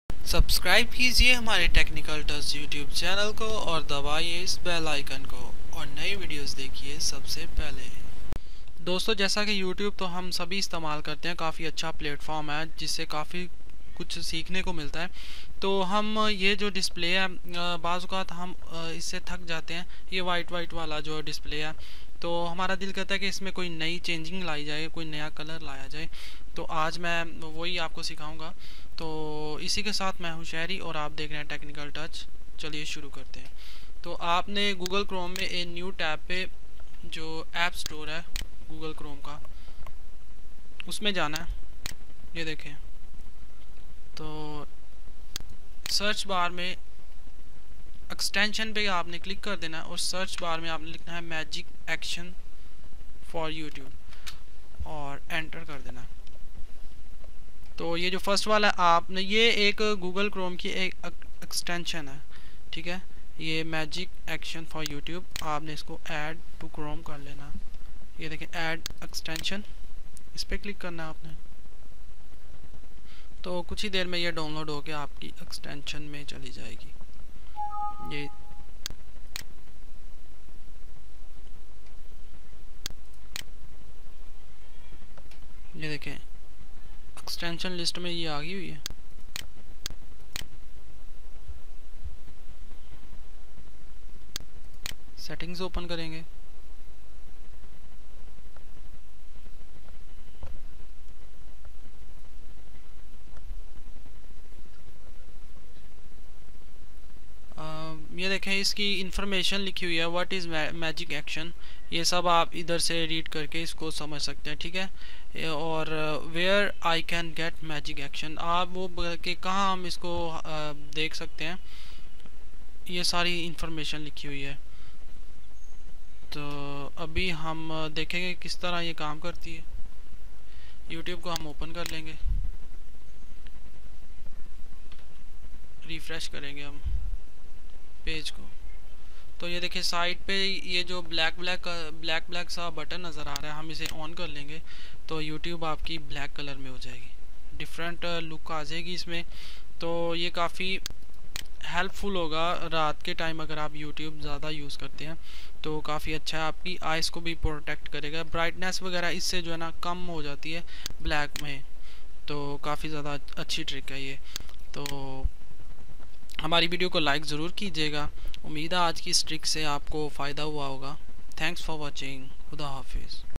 How To Change Youtube In Black Edition Easy Youtube Skins Technical Touch In Urdu
How To Change Youtube In Black Edition Easy Technical Touch In Urdu / Hindi

Hi Dosto aj ki is video mein me ap ko btaunga k Kesy ap youtube ka colour yan background ka rang badal skty hain to umeed hai ap ko ye video pasand aye ga.

◊ Hi YouTube , i'm  Sheri . . ▐
In Urdu

Allah HAfiz.

How To Protect Your Facebook From Hackers | FB Privacy Policy | FB Guides | Technical Touch In Urdu